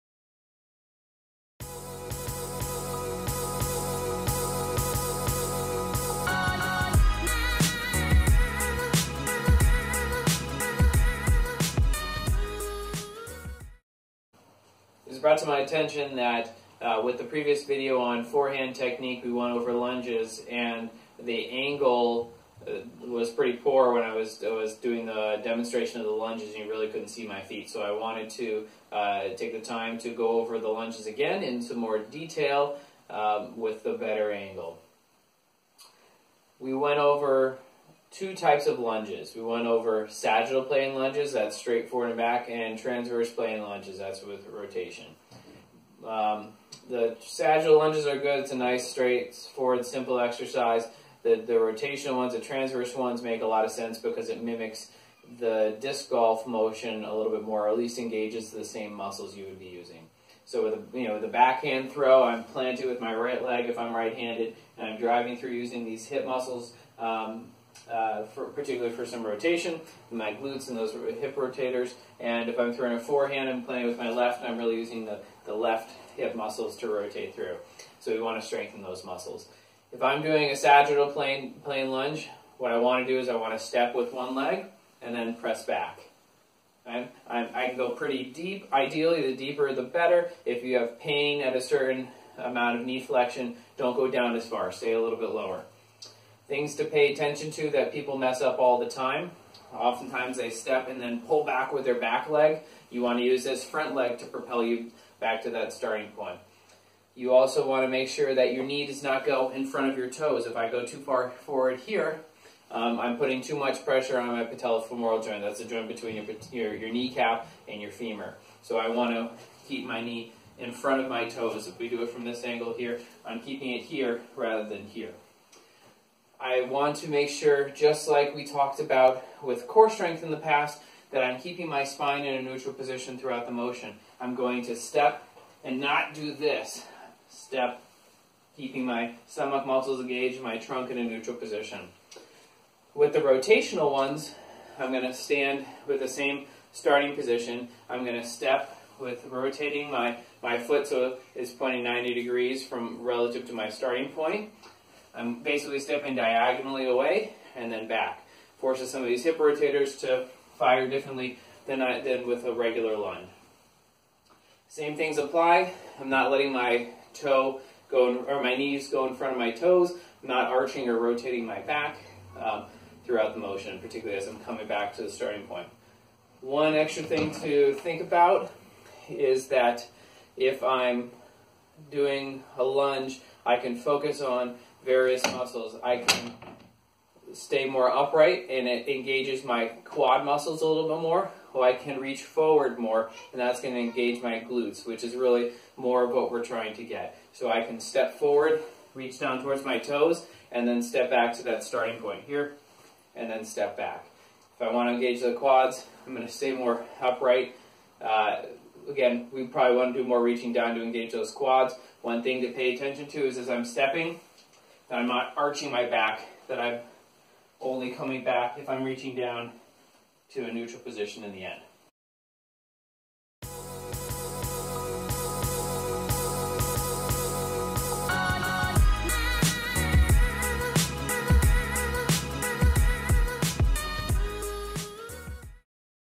Lunges for Disc Golf
Instruction in lunges specific to disc golf